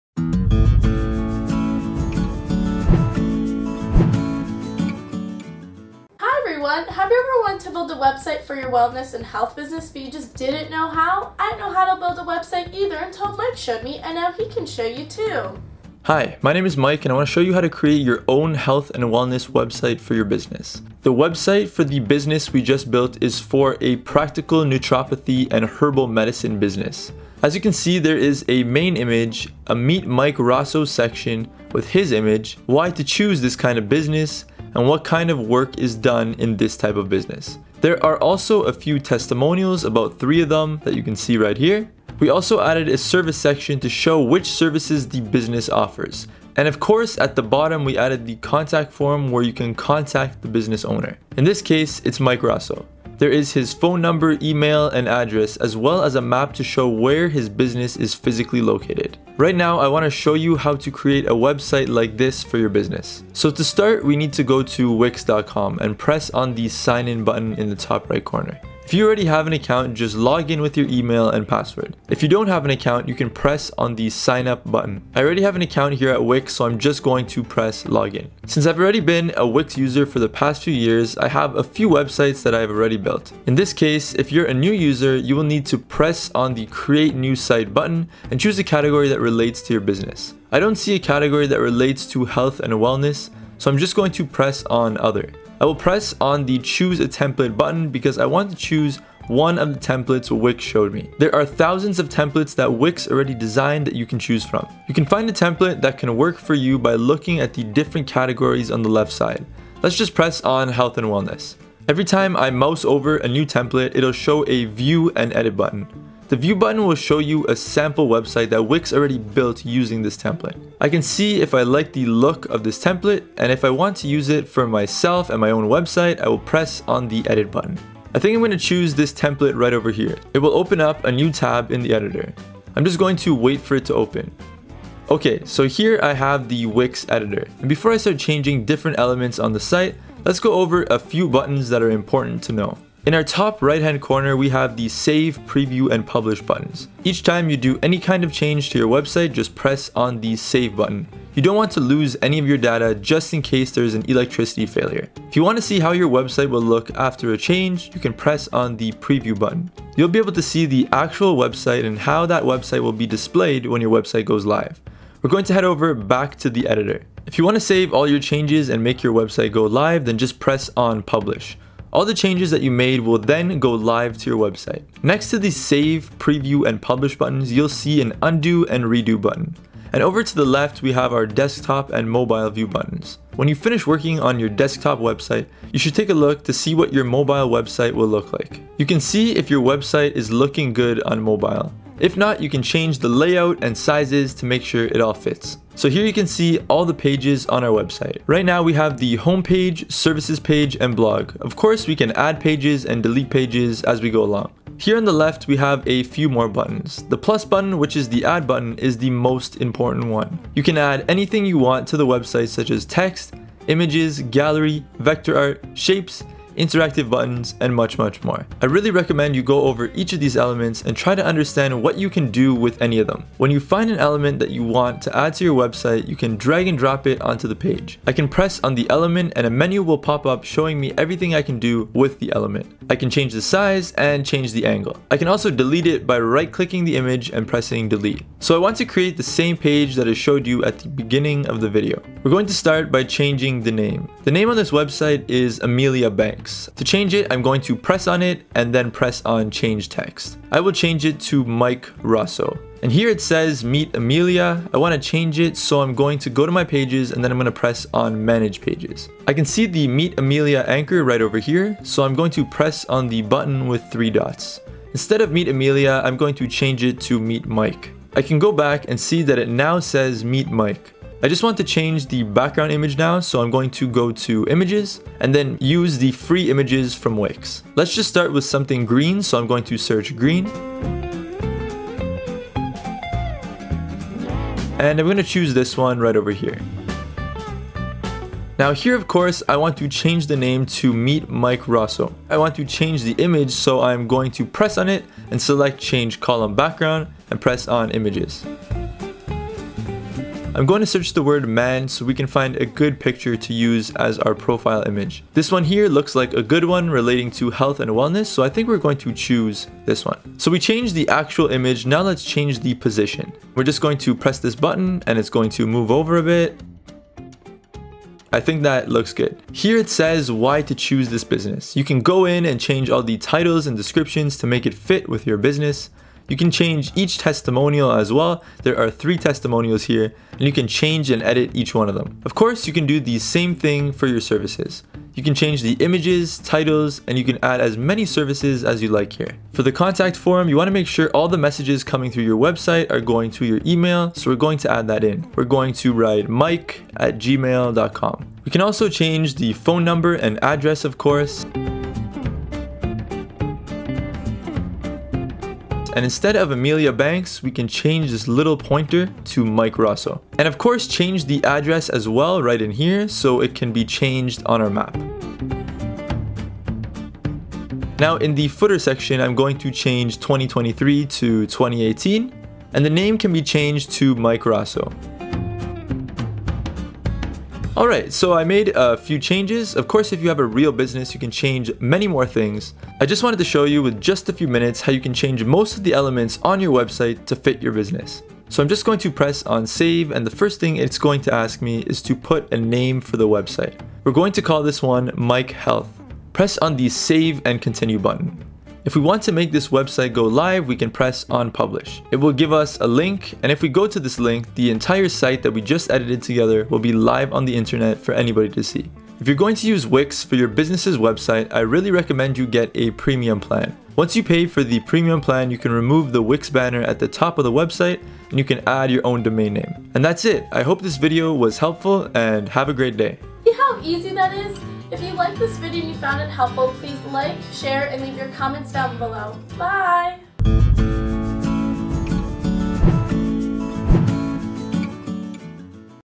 How to Create a Health and Wellness Website
In this tutorial, we show you how to create a beautiful website and online store using the Wix website builder.